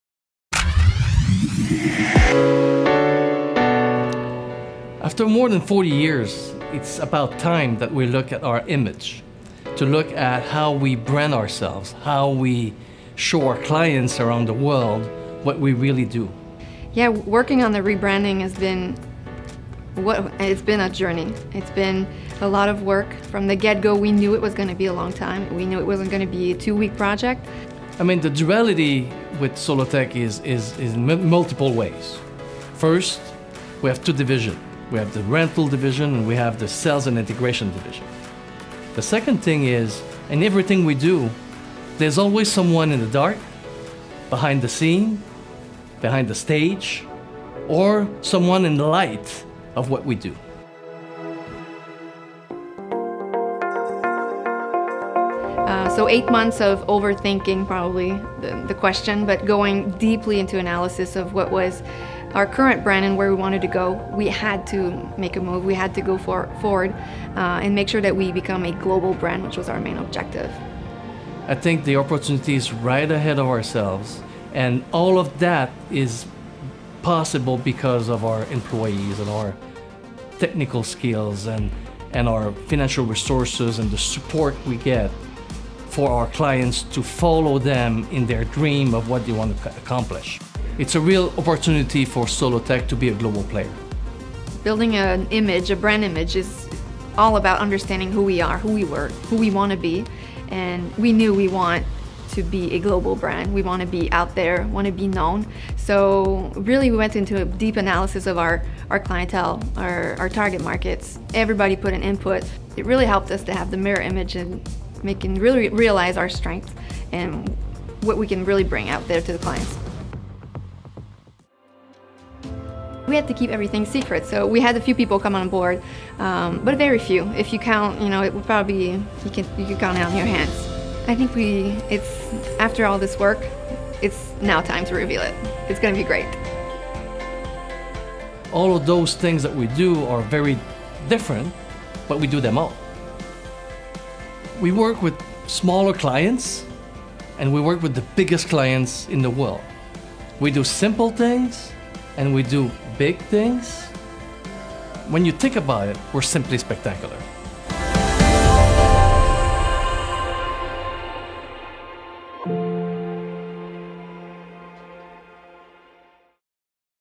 Behind the scenes of Solotech’s new brand
Solotech recently unveiled its new brand image, the new identity showcasing both the artists and the technicians, the onstage and the backstage, the cool colors and the warm hues. The new slogan “Solotech, Simply Spectacular” captures the company’s understanding of the duality needed to realize creative visions.